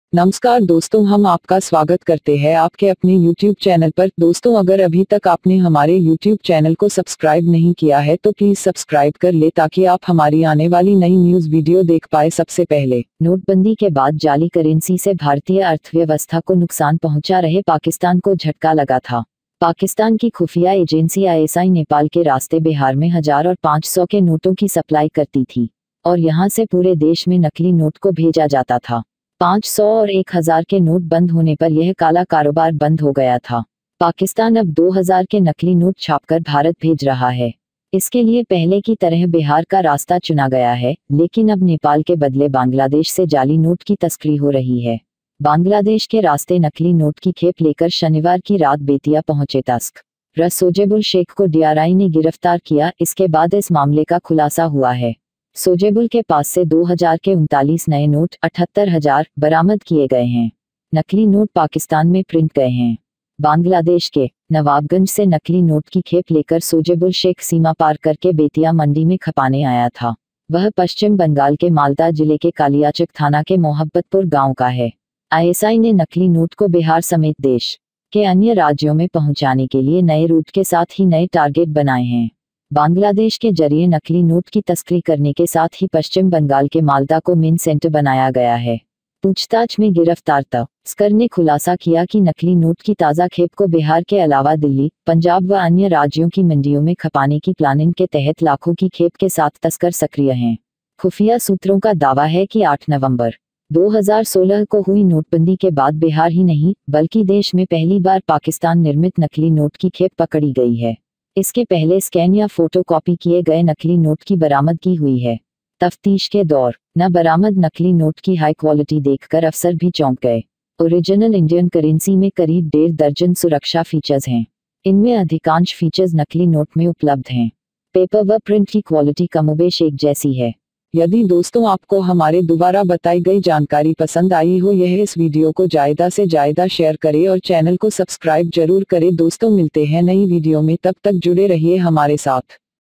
2000 रुपए का भी आ गया नकली नोट,ये हैं असली और जाली में अंतर|Difference Between Fake & Original 2000.
Difference Between Fake And Original 2000 Rupees Note.
2000 रुपए का भी आ गया नकली नोट, ये हैं असली और जाली में अंतर||

नोटबंदी के बाद जाली करेंसी से भारती की अर्थव्यवस्था को नुकसान पहुंचा रहे पाकिस्तान को झटका लगा था। पाकिस्तान की खुफिया एजेंसी आईएसआई नेपाल के रास्ते बिहार में हजार और पांच सौ के नोटों की सप्लाई करती थी और यहां से पूरे देश में नकली नोट को भेजा जाता था। 500 और 1000 के नोट बंद होने पर यह काला कारोबार बंद हो गया था। पाकिस्तान ने भेजा 2000 का नकली नोट
पाकिस्तान अब 2000 के नकली नोट छापकर भारत भेज रहा है। इसके लिए पहले की तरह बिहार का रास्ता चुना गया है, लेकिन अब नेपाल के बदले बांग्लादेश से जाली नोट की तस्करी हो रही है।
- बांग्लादेश के रास्ते नकली नोट की खेप लेकर शनिवार की रात बेतिया पहुंचे तस्कर सोजेबुल शेख को डीआरआई ने गिरफ्तार किया, इसके बाद इस मामले का खुलासा हुआ है।
- सोजेबुल के पास से 2 हजार के 39 नए नोट (78 हजार) बरामद किए गए हैं। नकली नोट पाकिस्तान में प्रिंट गए हैं।
- बांग्लादेश के नवाबगंज से नकली नोट की खेप लेकर सोजेबुल शेख सीमा पार करके बेतिया मंडी में खपाने आया था। वह पश्चिम बंगाल के मालदा जिले के कालियाचक थाना के मोहब्बतपुर गांव का है।
आईएसआई ने नए रूट के साथ ही नए टारगेट बनाए
- आईएसआई ने नकली नोट को बिहार समेत देश के अन्य राज्यों में पहुंचाने के लिए नए रूट के साथ ही नए टारगेट बनाए हैं।
- बांग्लादेश के जरिए नकली नोट की तस्करी करने के साथ ही पश्चिम बंगाल के मालदा को मेन सेंटर बनाया गया है।
- पूछताछ में गिरफ्तार तस्कर ने खुलासा किया कि नकली नोट की ताजा खेप को बिहार के अलावा दिल्ली, पंजाब व अन्य राज्यों की मंडियों में खपाने की प्लानिंग के तहत लाखों की खेप के साथ तस्कर सक्रिय हैं।
- खुफिया सूत्रों का दावा है कि 8 नवंबर, 2016 को हुई नोटबंदी के बाद बिहार ही नहीं, बल्कि देश में पहली बार पाकिस्तान निर्मित नकली नोट की खेप पकड़ी गई है। इसके पहले स्कैन या फोटो कॉपी किए गए नकली नोट की बरामदगी हुई है।
- तफ्तीश के दौरान बरामद नकली नोट की हाई क्वालिटी देख कर अफसर भी चौंक गए।
- ओरिजनल इंडियन करेंसी में करीब डेढ़ दर्जन सुरक्षा फीचर्स हैं। इनमें अधिकांश फीचर्स नकली नोट में उपलब्ध हैं। पेपर व प्रिंट की क्वालिटी कमोबेश एक जैसी है।